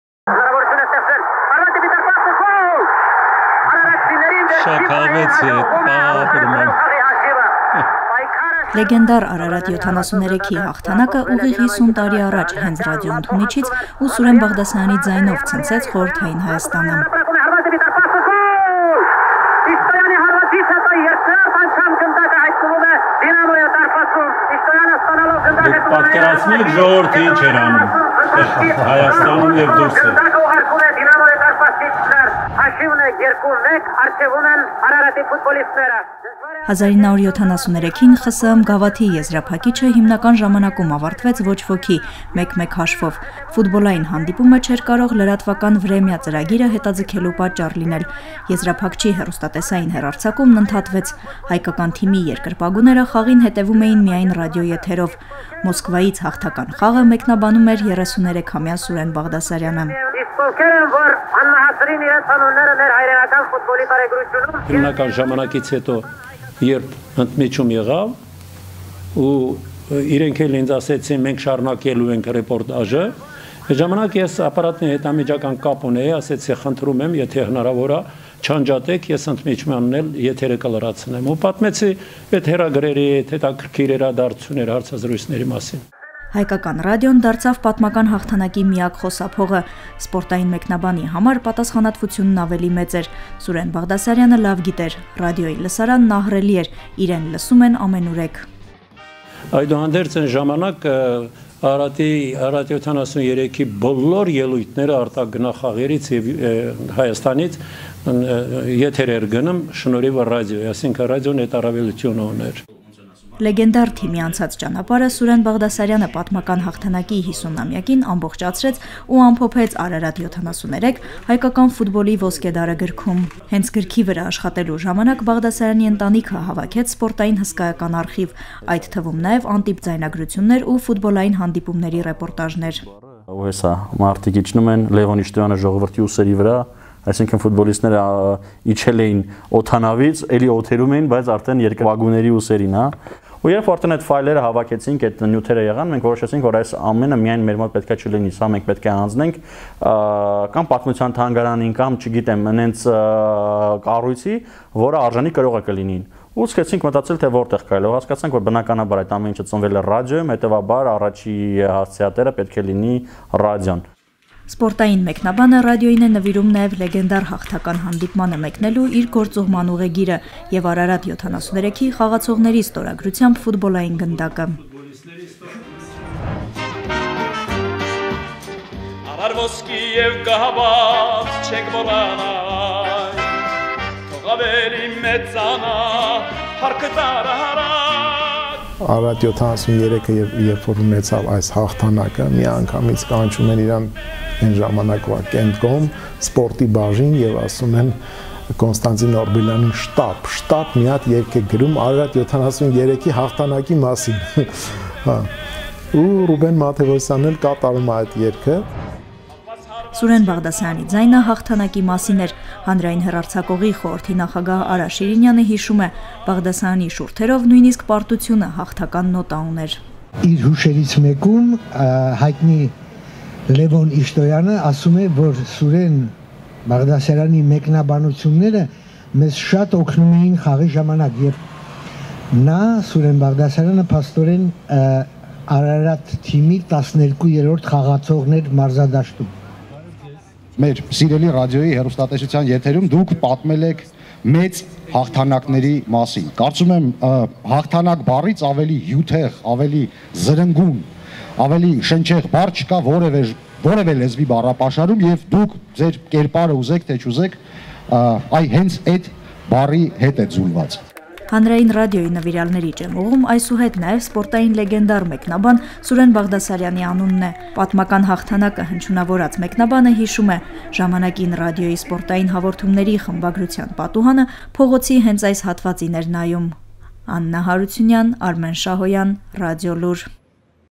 «Արարատ 73»-ի 12-րդ խաղացողը․ Սուրեն Բաղդասարյանի արխիվը կպահվի Հանրային ռադիոյի Ոսկե ֆոնդում
«Արարատ 73»-ի լեգենդար հաղթանակը ուղիղ 50 տարի առաջ հենց ռադիոընդունիչից ու Սուրեն Բաղդասարյանի ձայնով ցնցեց Խորհրդային Հայաստանը։ Հայկական ռադիոն դարձավ պատմական հաղթանակի միակ խոսափողը։ Պատմական հաղթանակը հնչունավորած մարզական մեկնաբանն իր կեսդարյա մասնագիտական արխիվը նվիրել է Հանրային ռադիոյի Ոսկե ֆոնդին, այն ռադիոյի, որտեղից ամեն ինչ սկսվել է։ Հանրային ռադիոյի նվիրյալների ճեմուղում այսուհետ նաև սպորտային լեգենդար մեկնաբան Սուրեն Բաղդասարյանի անունն է։